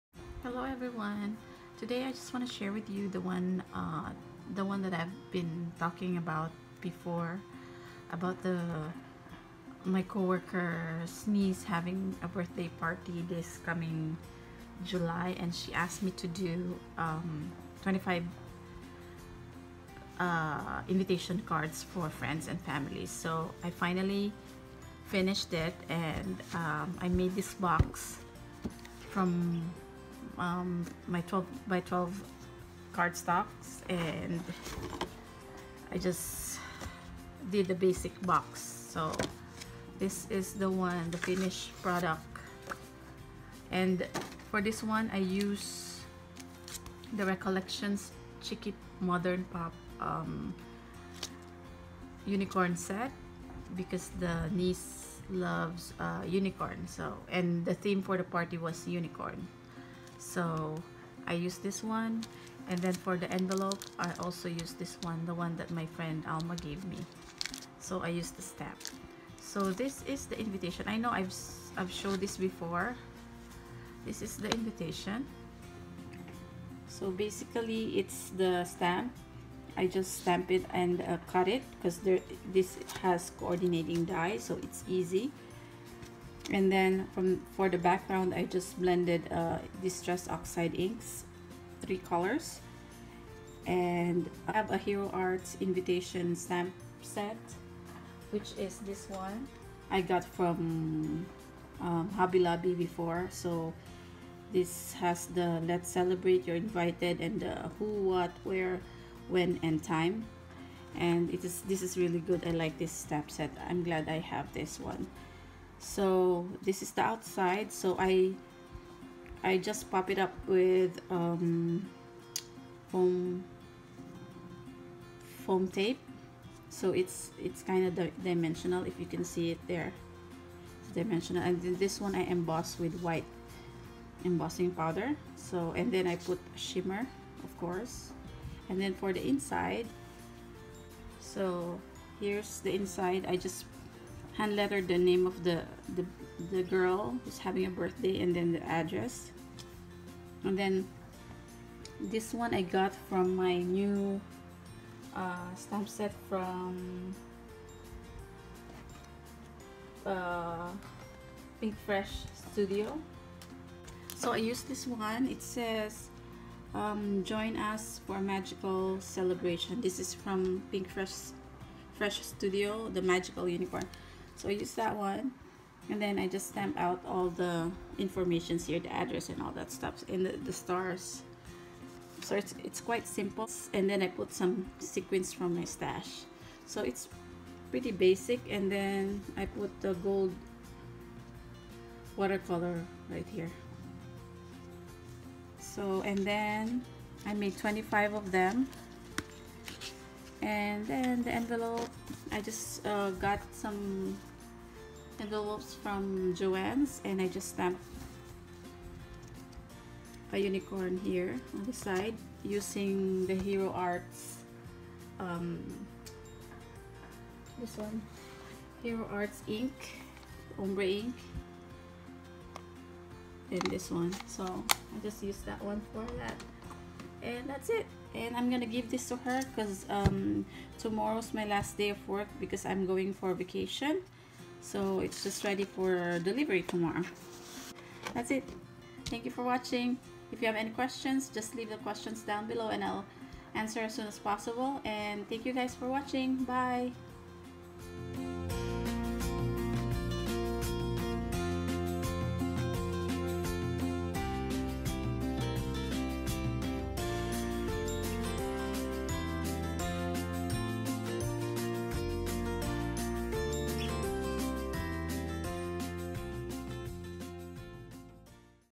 Project share: 🦄Unicorn themed handmade invitation cards🦄
Hi everyone! I just want to share the invitation cards I made, Unicorn themed, for my co-worker! Finally done and ready for delivery!
If you have any questions or want to know details just let me know❤️